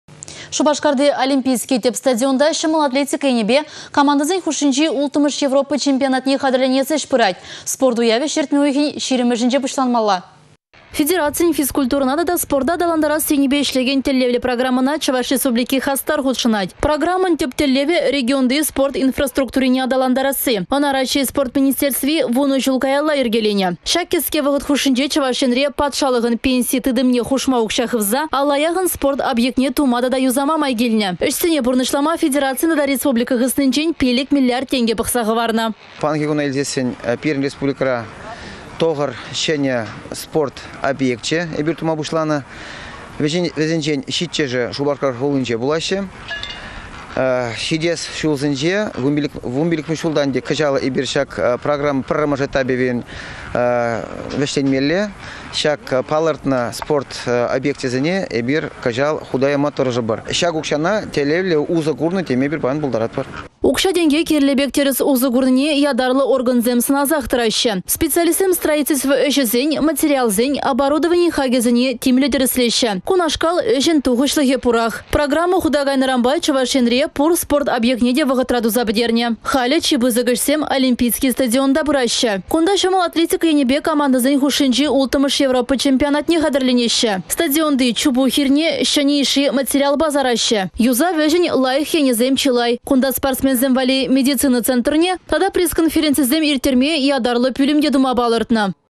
«Олимпийский» Европа чемпионатне хатĕрленет
Шупашкарти «Олимпийский» тĕп стадионта çăмăл атлетика енĕпе командăсен хушшинчи 6-мĕш  Европа чемпионатне хатĕрленес ĕç пырать. Спорт уявĕ çĕртме уйăхĕн 20-мĕшĕнче пуçланмалла.